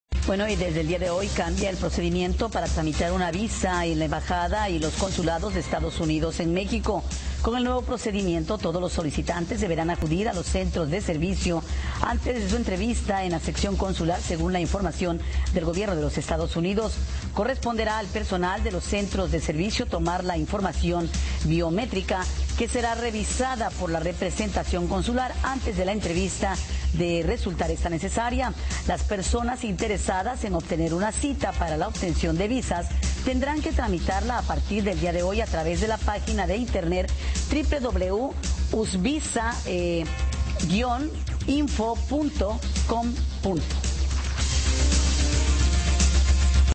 Obtención de VISA
Desde hoy cambia el procedimiento para tramitar una VISA en la Embajada y los Consulados de Estados Unidos en México